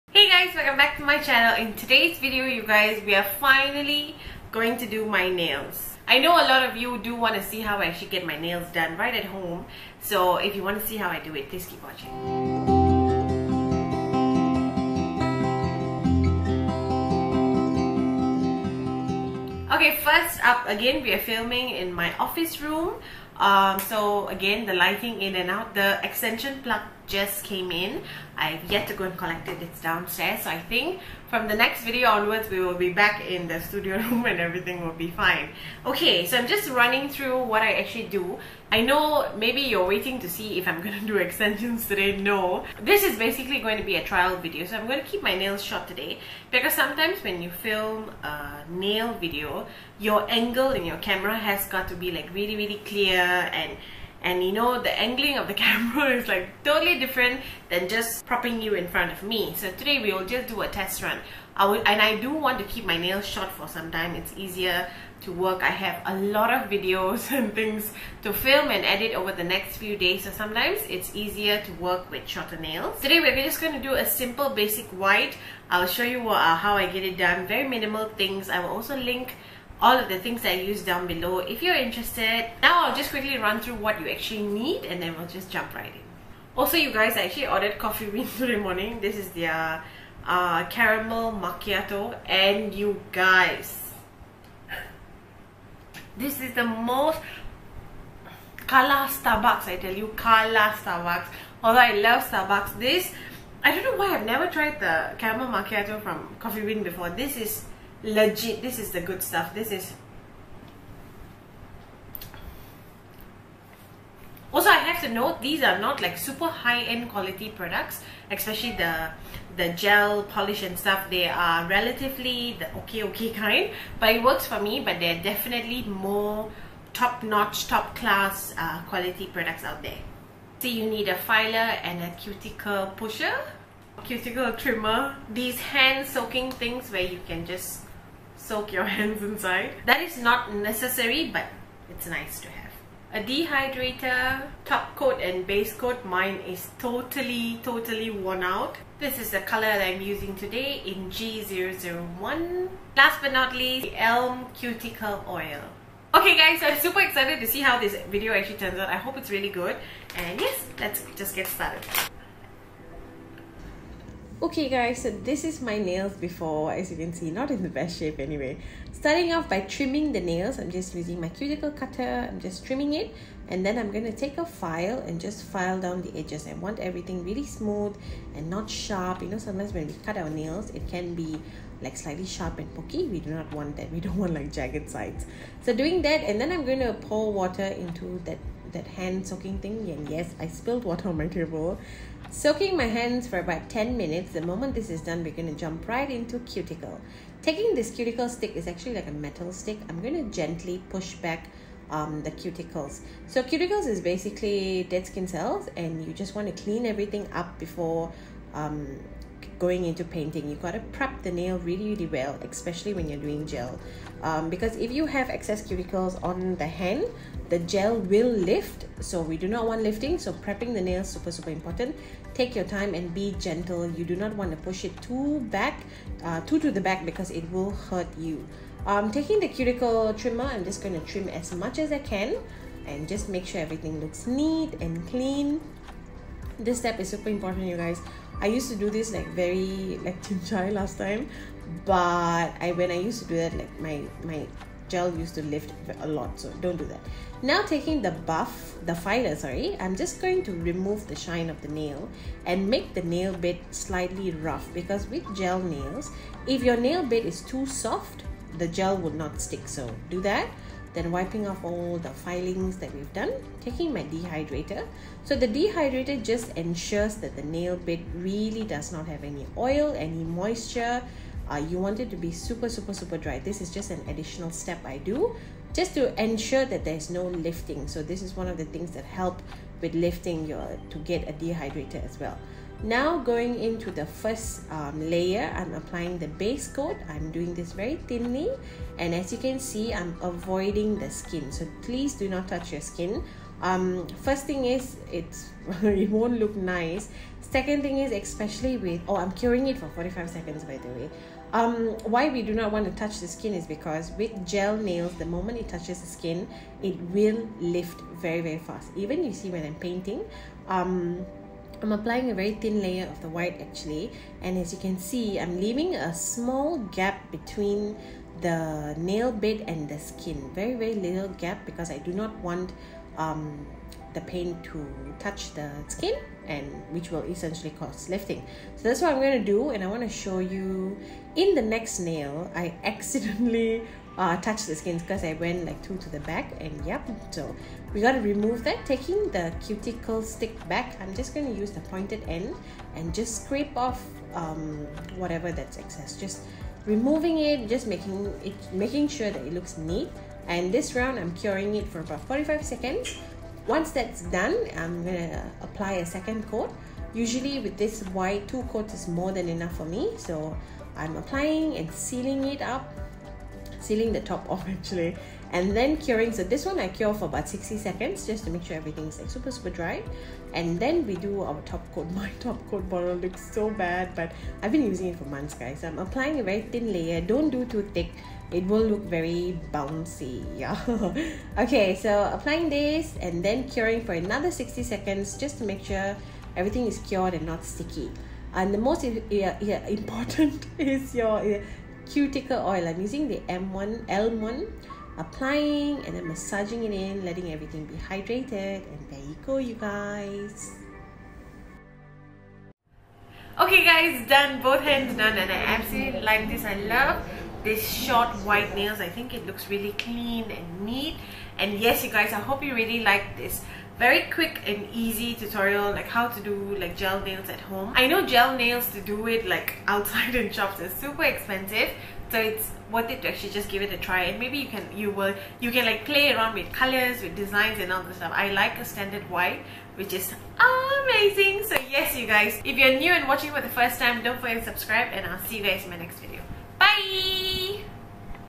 10 Minute Gel Manicure At Home :) | Items from Shopee Malaysia | Ash Edward
Hey guys! Welcome to my simple manicure at home using stuff I got from shopee Malaysia. this is so cost effective and super easy to do!